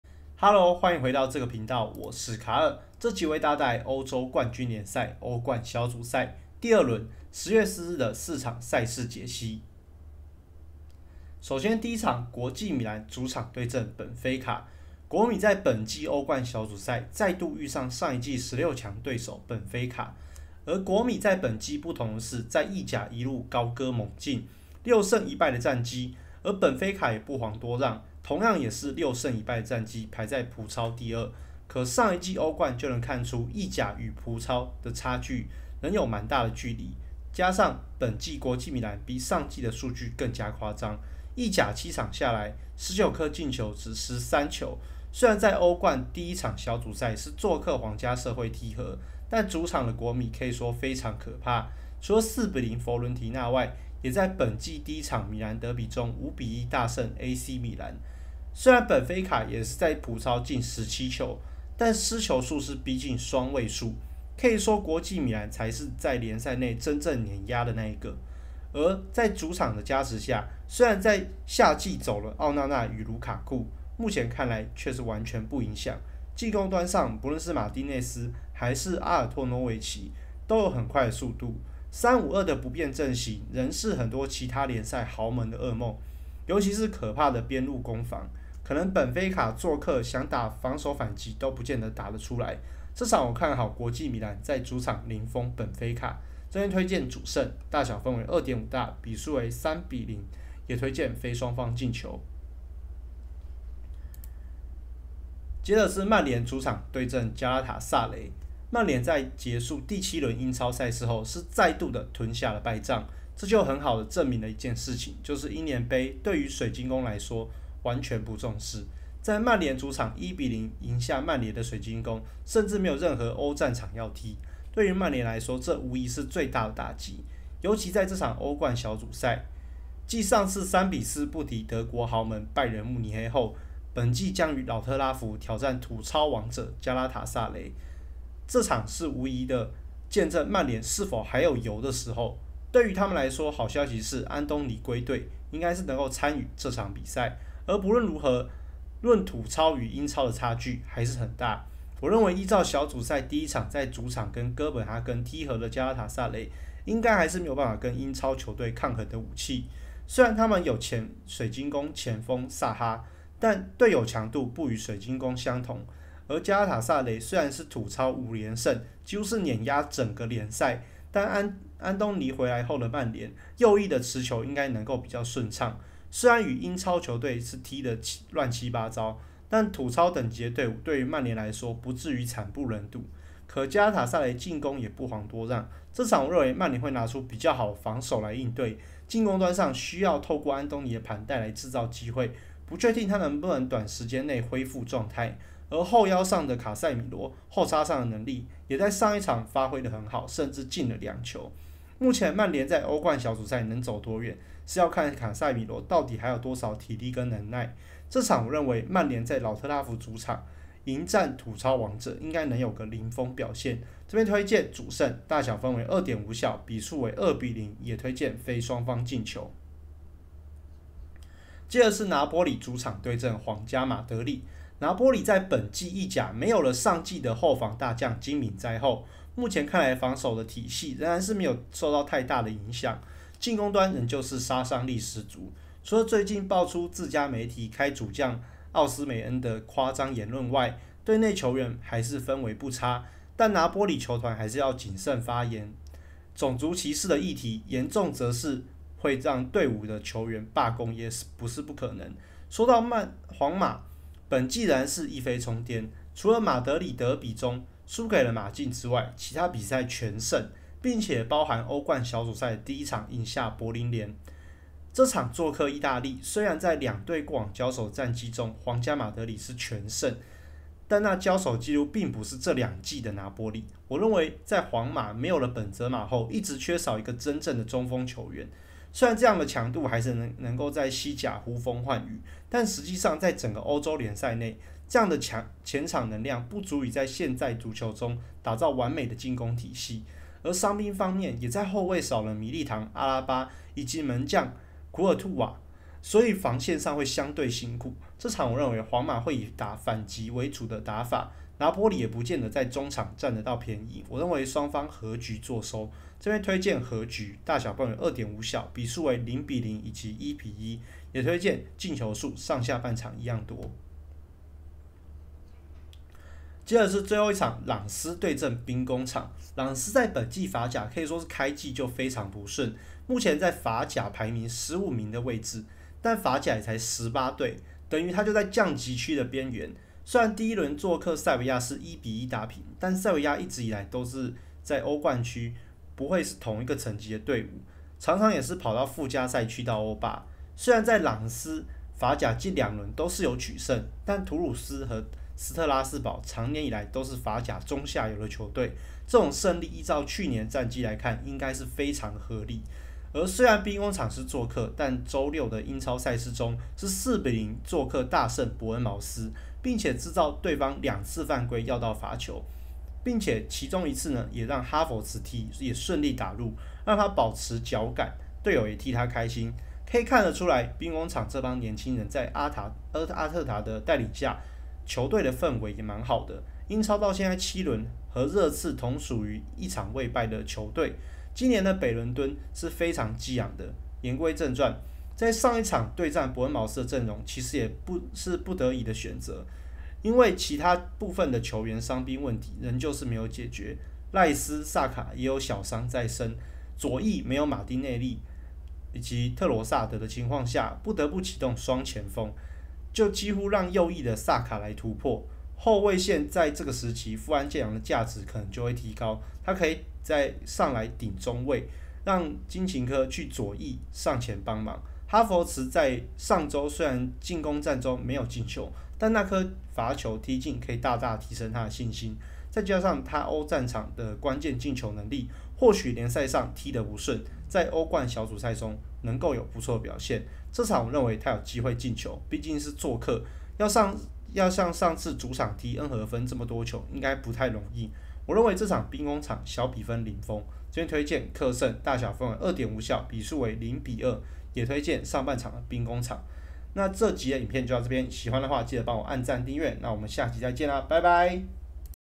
『歐洲冠軍聯賽』歐冠 小組賽 第二輪 10月04日 國際米蘭vs本菲卡 曼聯vs加拉塔薩雷 拿坡里vs皇家馬德里 朗斯vs兵工廠
請開啟CC字幕觀看!